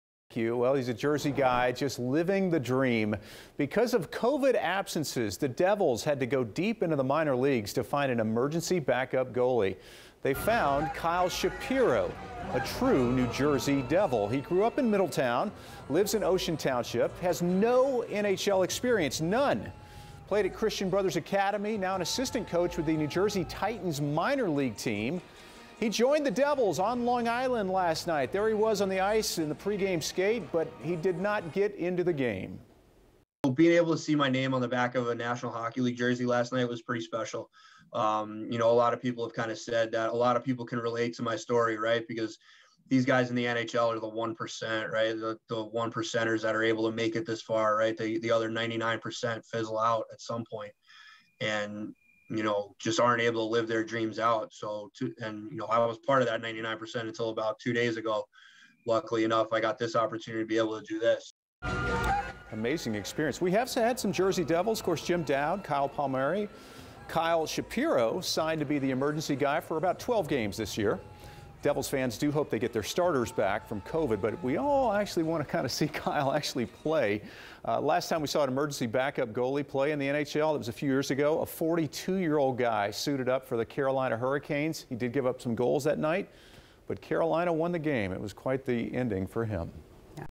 New Jersey assistant coach called up to play for Devils amid COVID-19 absences
The Devils were forced to go deep into the minor leagues to find an emergency backup goalie due to COVID-19 absences - choosing the assistant coach of the New Jersey Titans minor league team.